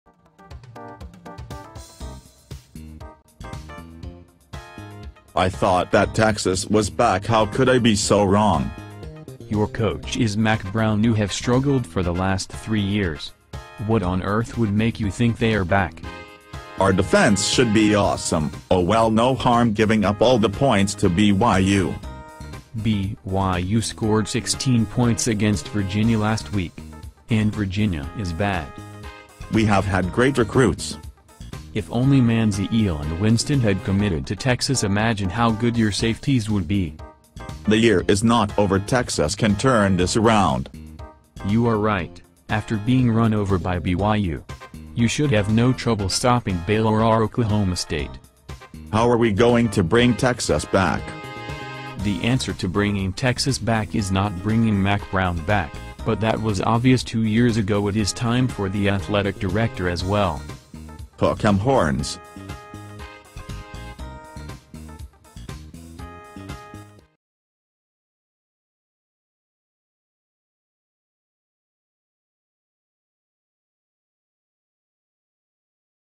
Texas Longhorn fan after losing to BYU Cougars
The Texas Longhorns were destroyed by the BYU Cougars as BYU ran right over them.  Why does Mack Brown have a job he and the AD should be fired.  This explains why Mack Brown should be fired